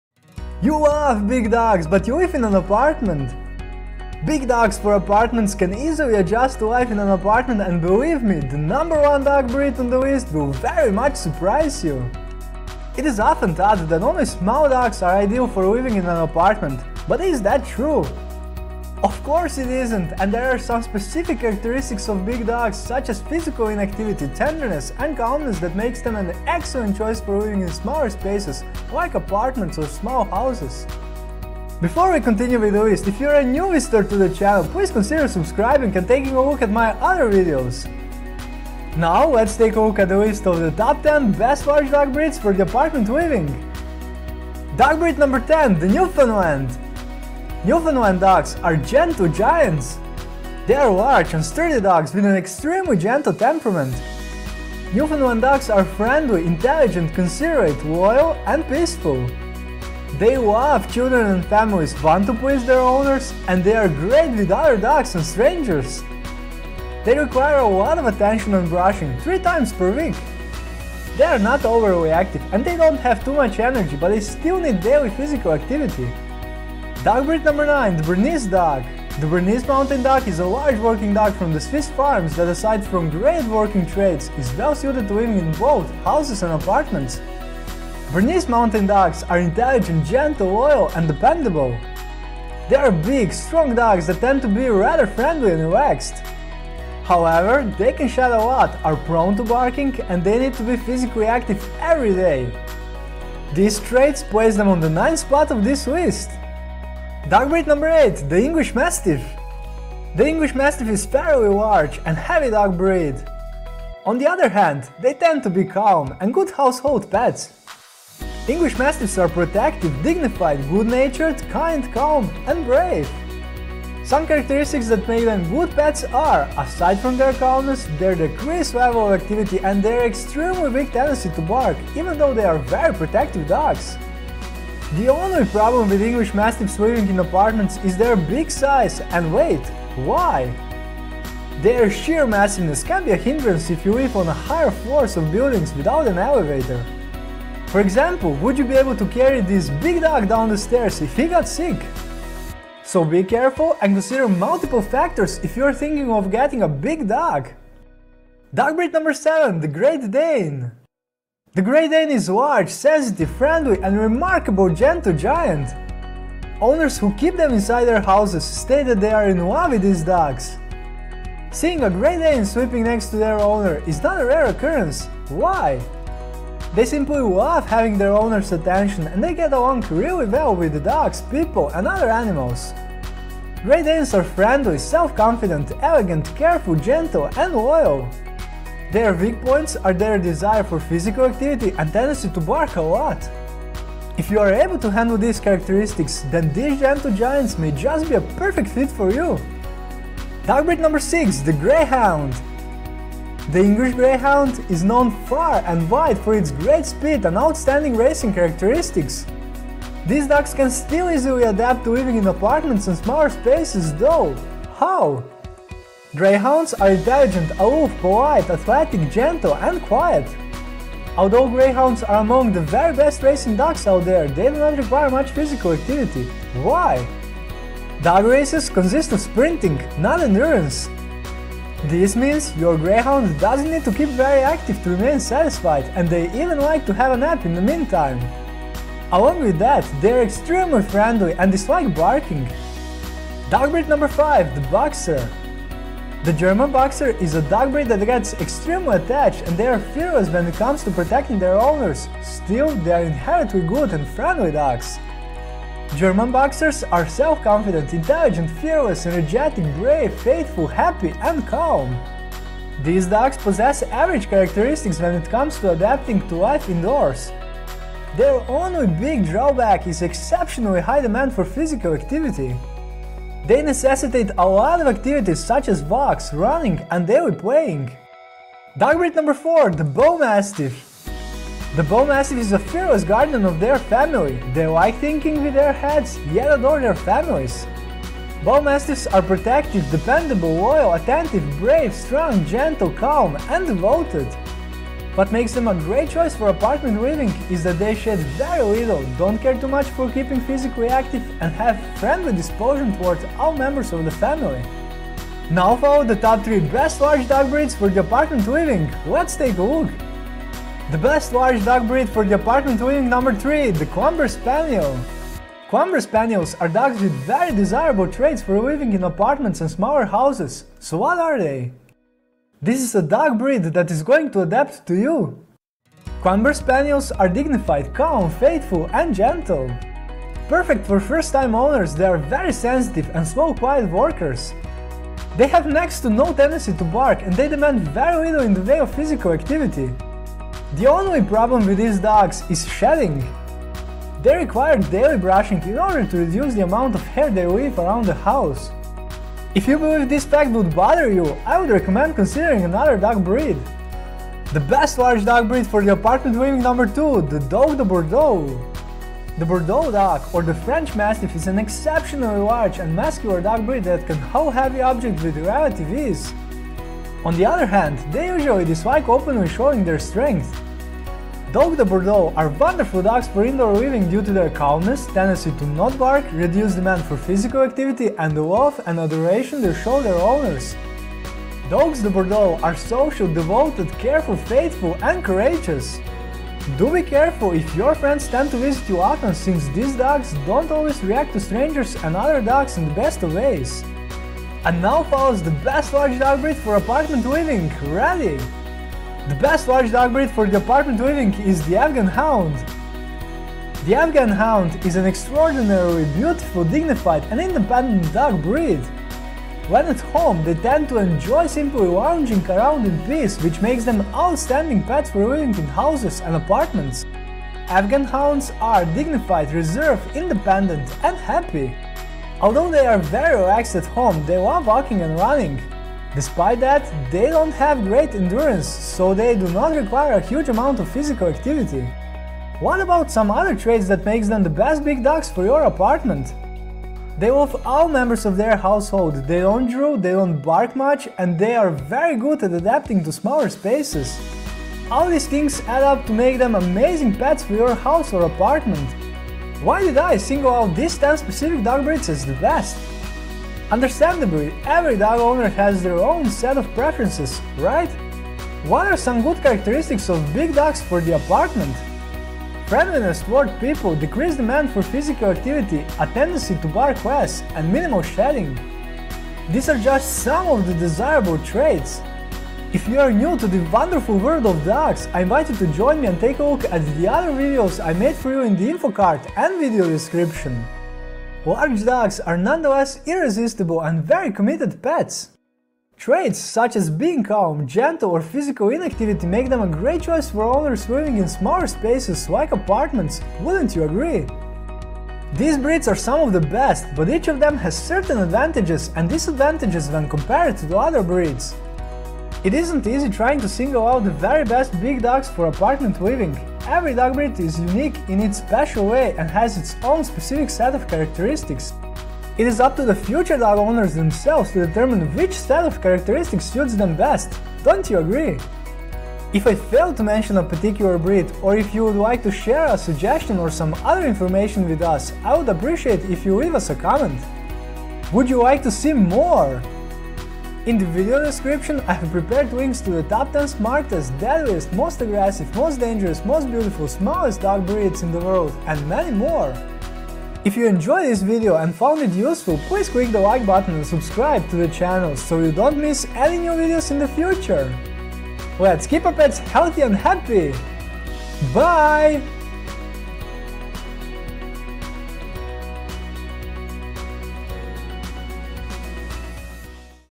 Big and Gentle Adorable Dogs!🐕 | YT Wonders
👉 Only small dogs are ideal for living in an apartment? So NOT true and here is why! List of the top 10 best large dog breeds for apartment living revels you ideal big dog breeds with all ideal characteristics.

✌️ Transcription ✌️

You love big dogs, but scared that they are harmful?

Let's take a look at the list of the Big & Gentle Dogs you can own

10. The Newfoundland.

Newfoundland Dogs are gentle giants.

They are large and sturdy dogs with an extremely gentle temperament.

They're not overly active and they don't have too much energy, but they still need daily physical activity.

9. The Bernese Dog.

The Bernese Mountain Dog is a large working dog from the Swiss farms that, aside from great working dog traits, is well suited to living in both houses and apartments.

However, they can shed a lot, are prone to barking and they need to be physically active every day.

8. The English Mastiff.

The English Mastiff is a fairly large and heavy dog breed.

Some characteristics that make them good pets are, aside from their calmness, their decreased level of activity and their extremely weak tendency to bark, even though they are very protective dogs.

7. The Great Dane.

The Great Dane is a large, sensitive, friendly and remarkably gentle giant.

Their weak points are their desire for physical activity and tendency to bark a lot.

If you are able to handle these characteristics, then these gentle giants may just be a perfect fit for you.

6. The Greyhound.

The English Greyhound is known far and wide for its great speed and outstanding racing characteristics.

These dogs can still easily adapt to living in apartments and smaller spaces though. How?

5. The Boxer.

The German Boxer is a dog breed that gets extremely attached, and they are fearless when it comes to protecting their owners.

These dogs possess average characteristics when it comes to adapting to life indoors.

4. The Bullmastiff.

The Bullmastiff is a fearless guardian of their family.

What makes them a great choice for apartment living is that they shed very little, don't care too much for keeping physically active, and have a friendly disposition toward all members of the family.

Now follow the top 3 best large dog breeds for apartment living, let's take a look!

3. The Clumber Spaniel.

Clumber Spaniels are dogs with very desirable traits for living in apartments and smaller houses. So what are they?

Perfect for first-time owners, they are very sensitive and slow, quiet workers.

They have next to no tendency to bark and they demand very little in the way of physical activity.

2. The Dogue De Bordeaux.

The Bordeauxdog, or the French Mastiff, is an exceptionally large and muscular dog breed that can haul heavy objects with relative ease.

Dogues de Bordeaux are wonderful dogs for indoor living due to their calmness, tendency to not bark, reduced demand for physical activity, and the love and adoration they show their owners.

And now follows the best large dog breed for apartment living. Ready?

1. The Afghan Hound.

The Afghan Hound is an extraordinarily beautiful, dignified and independent dog breed.

When at home, they tend to enjoy simply lounging around in peace, which makes them outstanding pets for living in houses and apartments.

They love all members of their household, they don't drool, they don't bark much and they are very good at adapting to smaller spaces.

All these things add up to make them amazing pets for your house or apartment.

It isn't easy trying to single out the very best big dogs for apartment living.

Every dog breed is unique in its special way and has its own specific set of characteristics.

It's up to the future dog owners themselves to determine which set of characteristics suits them best.

Take care!

TOP 10 Big Dog Breeds For Apartments!